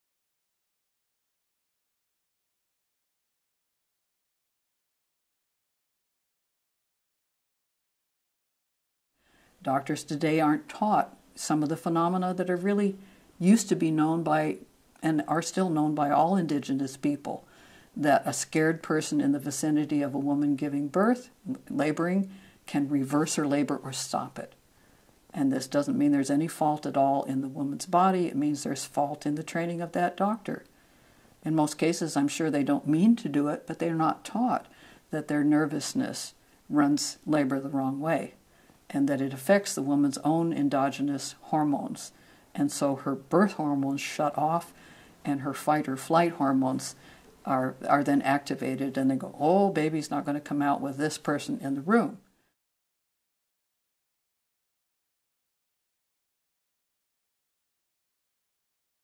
Ina May Gaskin on fear in childbirth
- Ina May Gaskin, author midwife & birth activist, talks about the fear in childbirth. One World Birth is a new FREE online video resource with the aim of sharing evidence-based information from the world's leading birth experts.